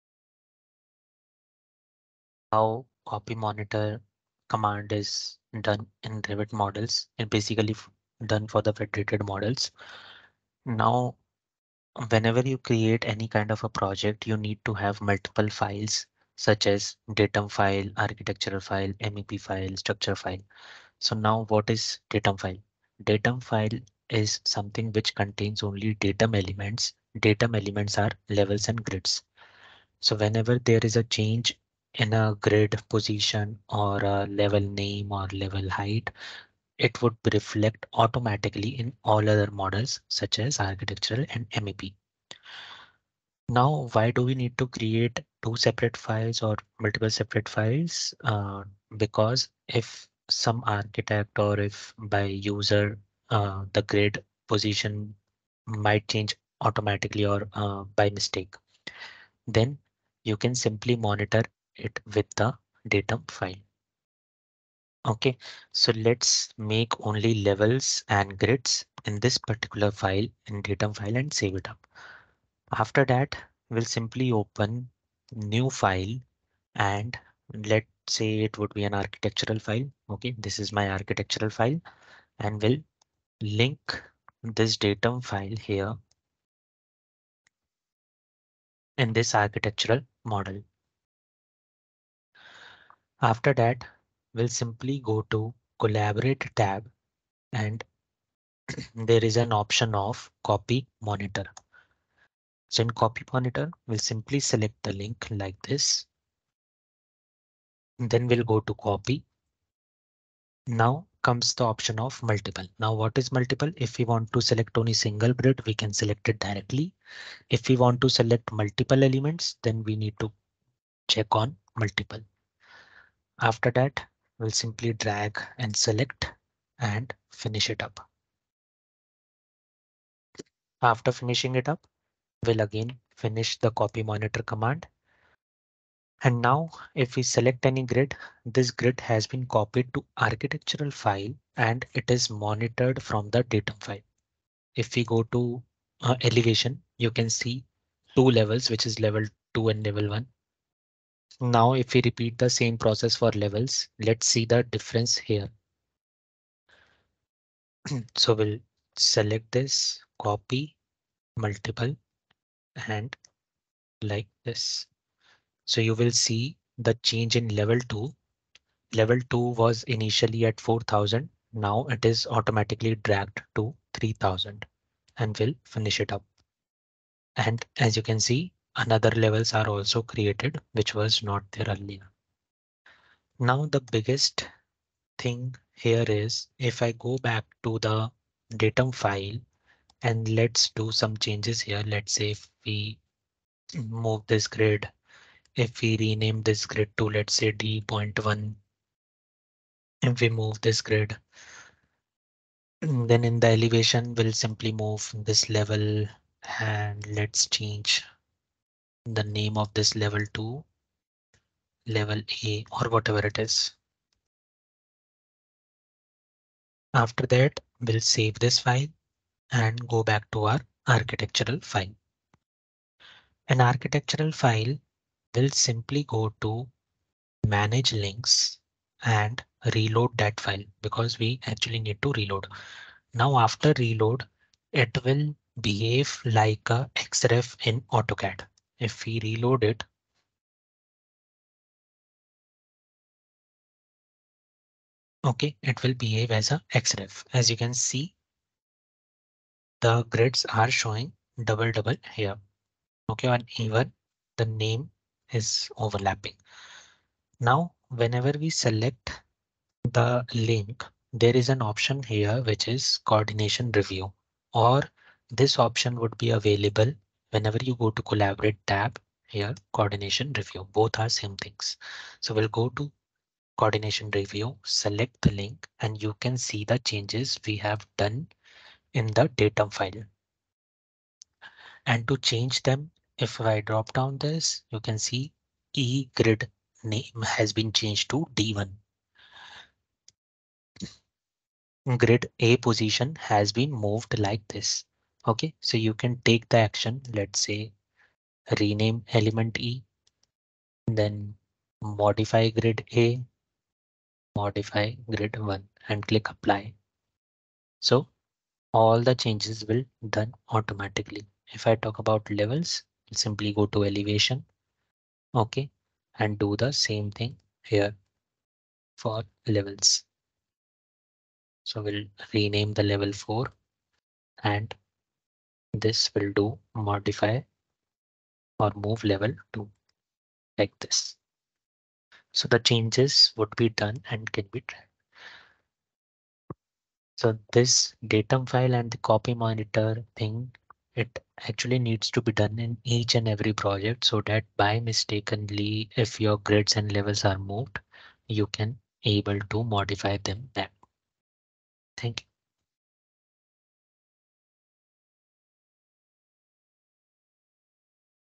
Copy Monitor Command in Revit
How copy monitor command helps to manage the datum elements in Revit